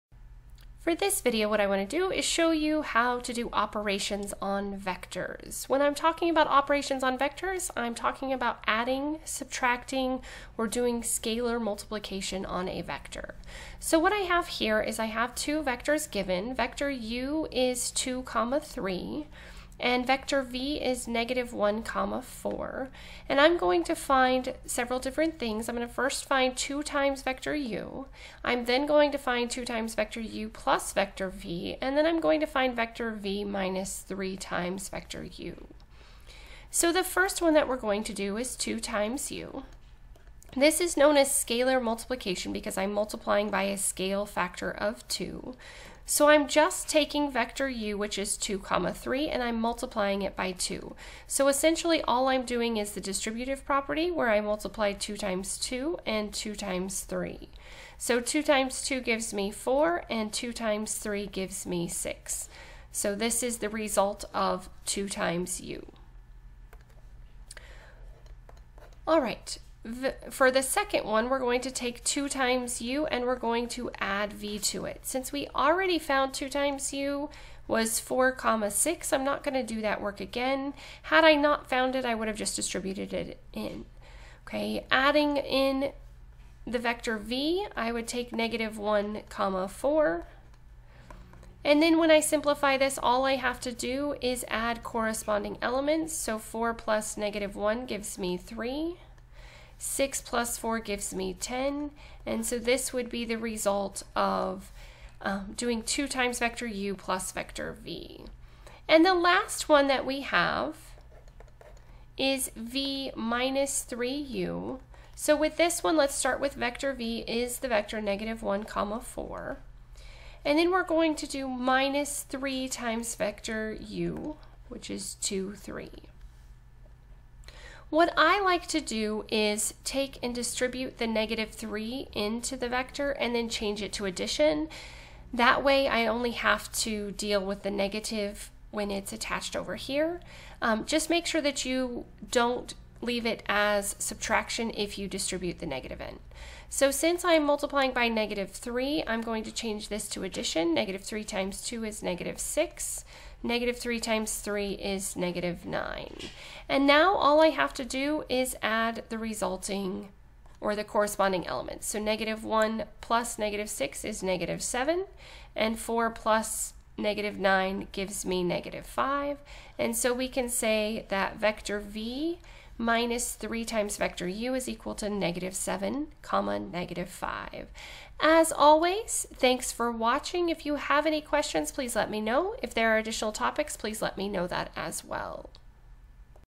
Operations on Vectors
This video shows how to perform operations on vectors. Operations on vectors include scalar multiplication, adding and subtracting vectors.